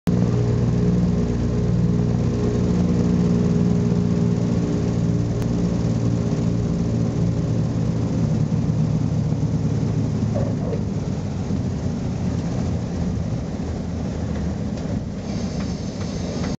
DC3-Landing.MOV
DC3 Landing at Ardmore, Auckland. On Runway 03 (ie facing a compass direction of 30 degrees aka NNE)